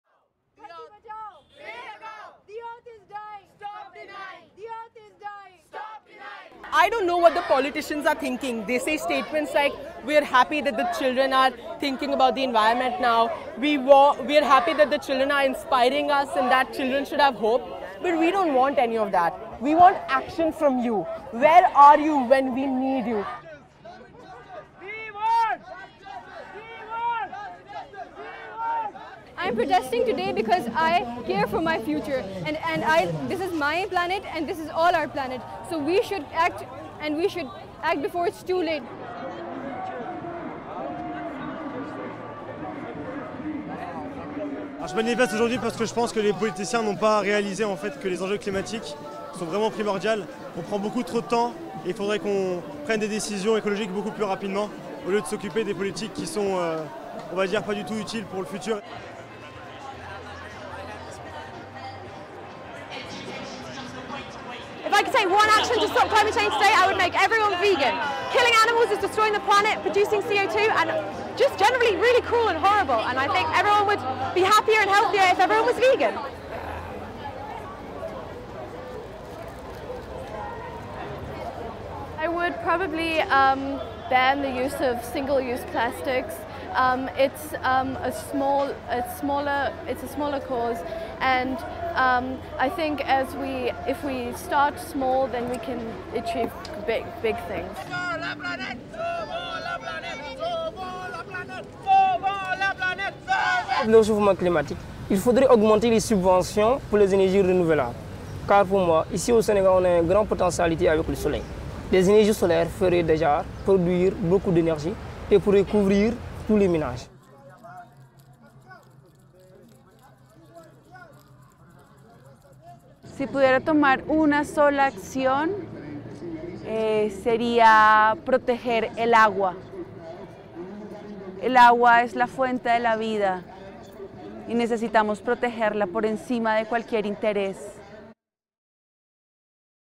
Globaler Klimastreik: Das sagen Klimaschützer in aller Welt
In aller Welt gehen Menschen gegen den Klimawandel auf die Straße. Was sind ihre Zielen und Beweggründe?